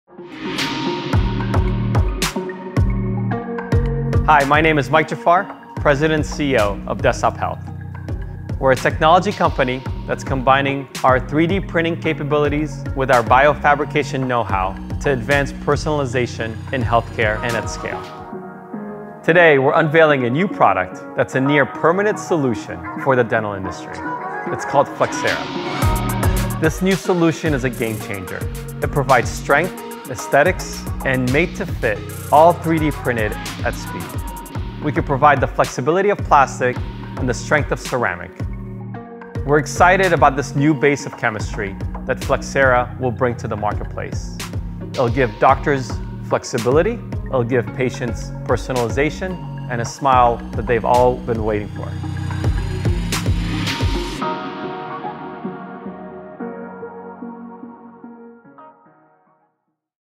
Introducing Flexcera™
Dentures Just Got an Upgrade 💪🏼
Introducing Flexcera™, a new hybrid ceramic resin for the fabrication of beautiful, functional dentures with ceramic-like strength.